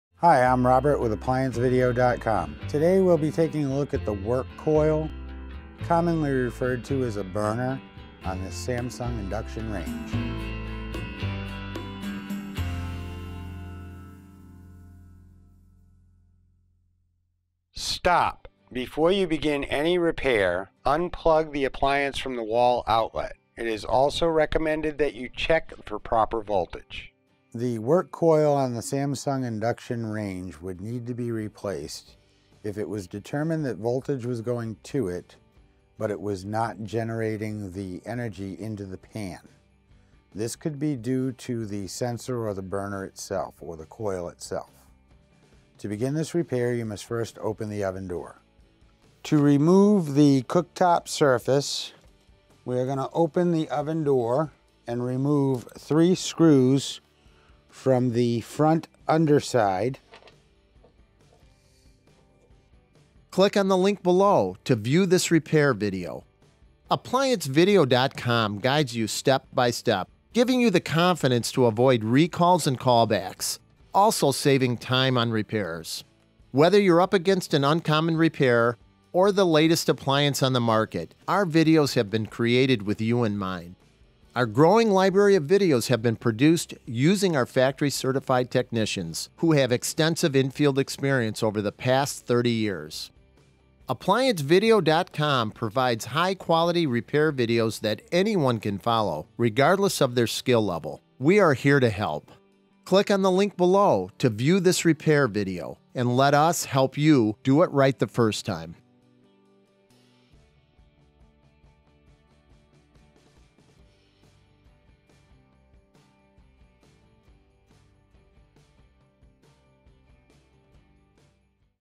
Samsung Stove - No Power - Work Coil Repair and Diagnostic
This is a preview of a video that shows you how to Diagnose and Repair a *NE595N0PBSR Samsung Stove DG96-00193B Work Coil*
*Symptoms may include:* No Power or Surface element not working